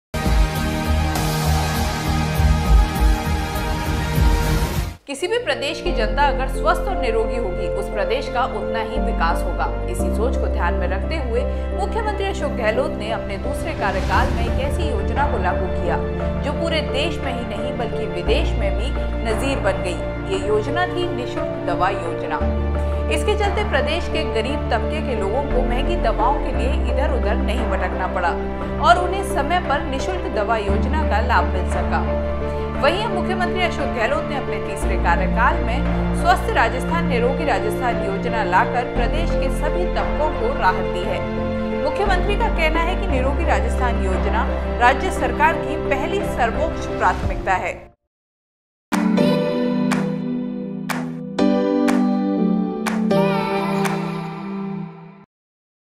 मुख्यमंत्री अशोक गहलोत का पहला लक्ष्य
किसी भी प्रदेश की जनता अगर स्वस्थ और निरोगी होगी। उस प्रदेष का उतना ही विकास होगा। इसी सोच को ध्यान में रखते हुए मुख्यमंत्री अषोक गहलोत ने अपने दूसरे कार्यकाल में एक ऐसी योजना को लागू किया जो पूरे देश में ही नहीं विदेष में भी एक नजीर बना गया। ये योजना थी निःषुल्क दवा योजना। इसके चलते प्रदेश  के गरीब तबके के लोगों को महंगी दवाओं के लिए इधर-उधर नहीं भटकना पड़ा और उन्हें समय पर निःषुल्क दवा योजना का लाभ मिला। वहीं अब मुख्यमंत्री अशोक गहलोत ने अपने तीसरे कार्यकाल में स्वस्थ राजस्थान निरोगी राजस्थान योजना लाकर प्रदेश के सभी तबकों को राहत दी है। मुख्यमंत्री का कहना है कि निरोगी राजस्थान योजना राज्य सरकार की पहली सर्वोच्च प्राथमिकता है।